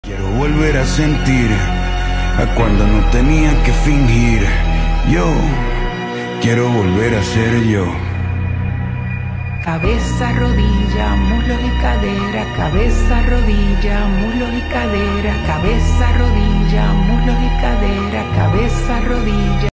no Winnie que te paso
por cierto la primera foto es de Winnie the Pooh bloody honey, y decirme sobre uqe personajes os gustaría que este tipo de videos